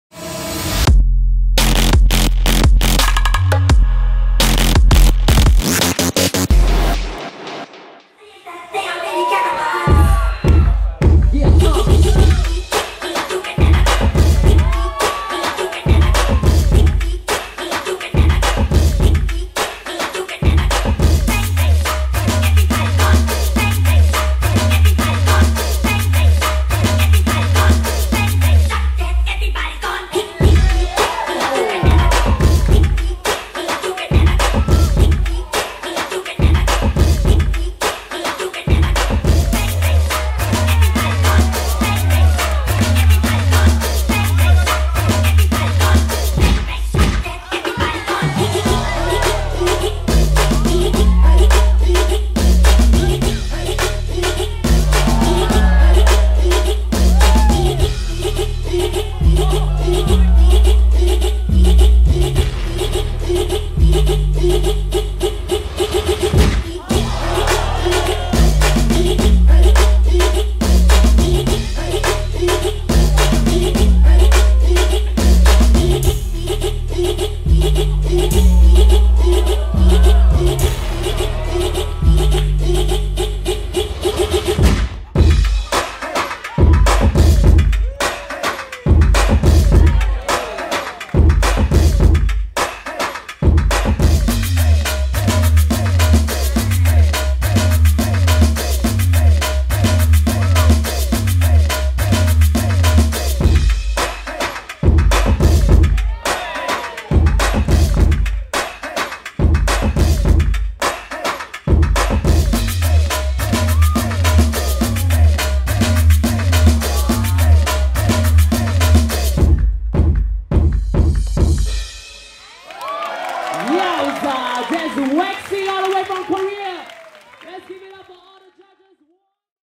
Waackxxxy (KOR) | Judge Showcase | AUDC Singapore 2016 | RPProds
Asia University Dance Championship (AUDC) Singapore Qualifiers

AUDC is a new competition platform for tertiary institutions of all over Asia to participate in. The 11 qualifying cities include: Shanghai, Beijing, Chengdu, Kunming, Guangzhou, Wuhan, Nanjing, Osaka, Seoul, Taipei and Singapore. The champions of each qualifying city will be sponsored to the AUDC Grand Finals, to be held in Shanghai on 26 November 2016.

AUDC is open to students of tertiary institutions (all local universities and polytechnics apply).

Judges:
Kayte Willis (Blaqueneez / Havoc)
Rachel (Scrachmarcs / Alt Ctrl)
Lum Lum (O Crew / Deep Ft / Ragga Waka)
Jojo Hung (CJ Crew)
Waackxxxy (Prepix)